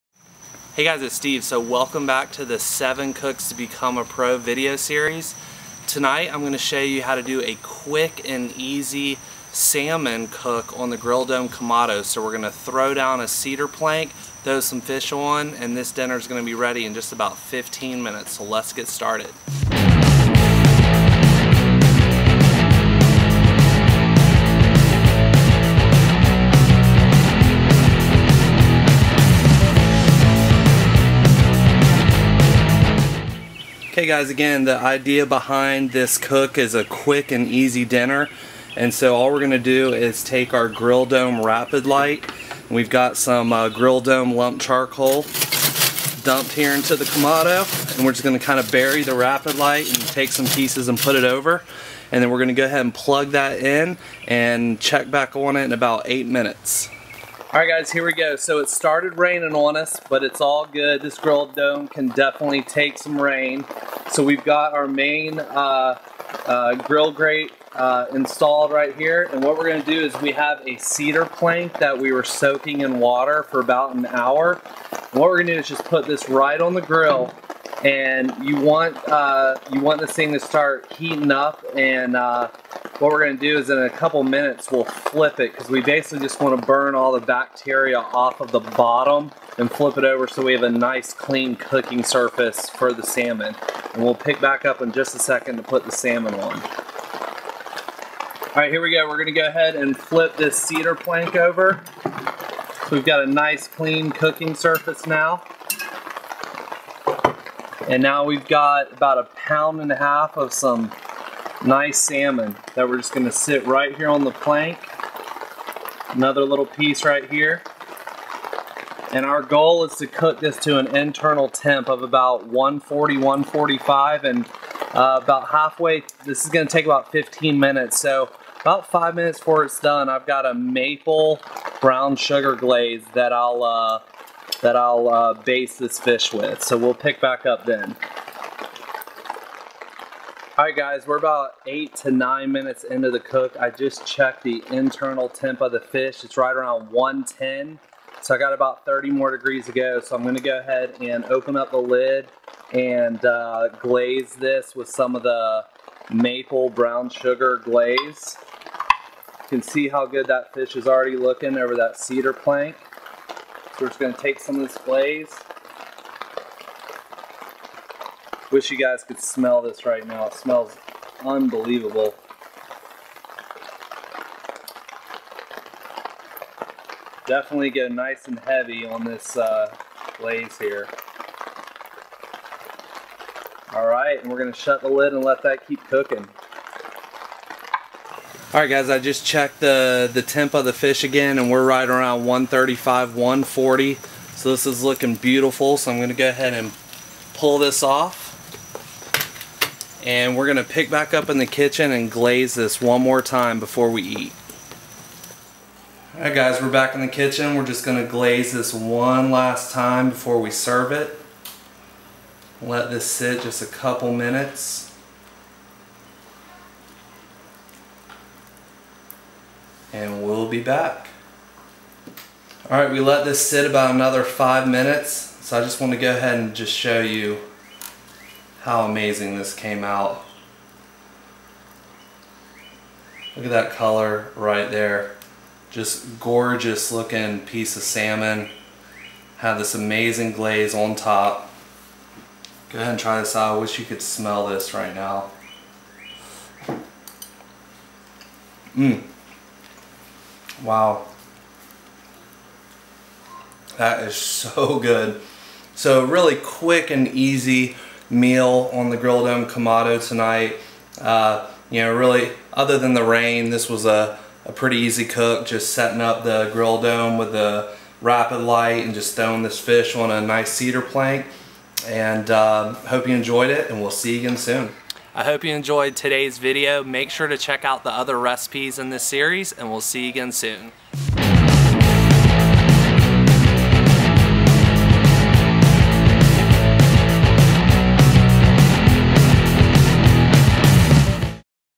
Grill Dome Kamados - 7 Cooks to Become a PRO - Grilled Salmon
We continue our 7 Cooks To Become A Pro series by demonstrating the Grill Dome Rapid-Lite and walking through how quick and easy it is to grill up some amazing salmon on the Grill Dome kamado.